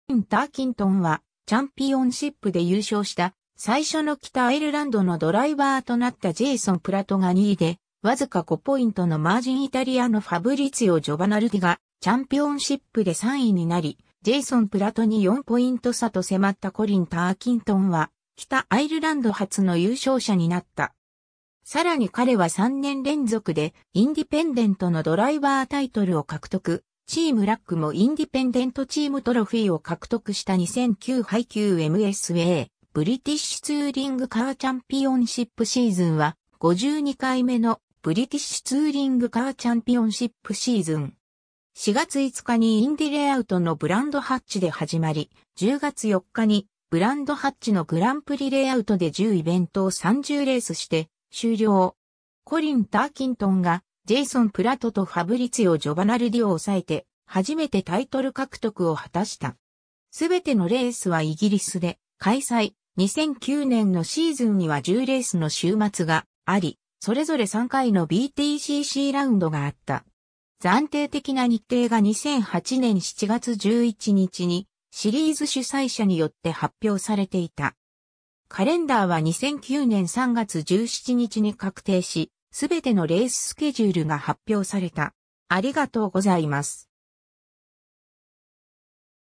2009年のイギリスツーリングカー選手権
コリン・ターキントンは、チャンピオンシップで優勝した最初の北アイルランドのドライバーとなった ジェイソン・プラトが2位で、わずか5ポイントのマージン イタリアのファブリツィオ・ジョバナルディがチャンピオンシップで3位になり、ジェイソン・プラトに4ポイント差と迫った コリン・ターキントンは、 北アイルランド初の優勝者になった。
さらに彼は3年連続でインディペンデントのドライバータイトルを獲得、 チームRACもインディペンデントチームトロフィーを獲得した 2009 HiQ MSAブリティッシュツーリングカーチャンピオンシップシーズン（The 2009 HiQ MSA British Touring Car Championship season）は、52回目のブリティッシュツーリングカーチャンピオンシップ （BTCC）シーズン。
 4月5日にインディレイアウトのブランドハッチで始まり、10月4日にブランドハッチのグランプリレイアウトで10イベントを30レースして終了。
 コリン・ターキントンが ジェイソン・プラトとファブリツィオ・ジョバナルディを抑えて、初めてタイトル獲得を果たした。
 すべてのレースはイギリスで開催、2009年のシーズンには10レースの週末があり、それぞれ3回のBTCCラウンドがあった。
暫定的な日程が2008年7月11日にシリーズ主催者によって発表されていた。
 カレンダーは2009年3月17日に確定し、すべてのレーススケジュールが発表された。